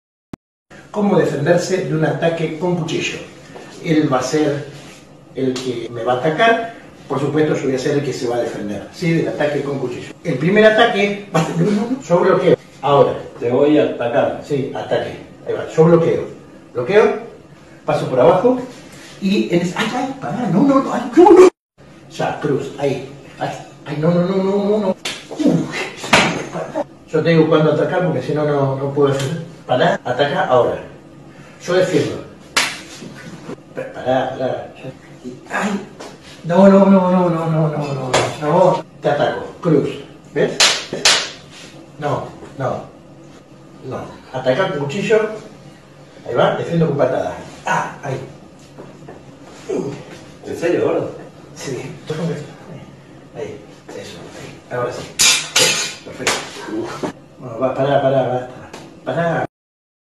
Legit Self Defense Skills
Dude disarms man with spatula!